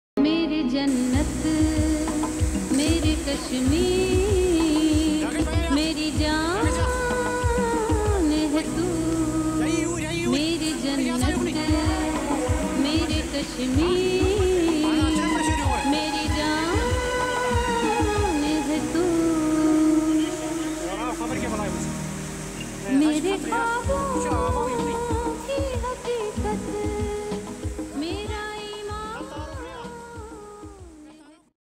Kashmir Flood Devastation
Handicapped and mentally challenged individuals are the ones who are most affected in times of dire need and tragedy due to any calamity. They are not able to effectively recuperate and get the help they need. Therefore, the weight is upon our shoulders to care for them. Please donate to the Voluntary Medicare Society, a nonprofit organization that has even been recognized by the CNN-IBN Real Heroes foundation under the category of Health & Disability.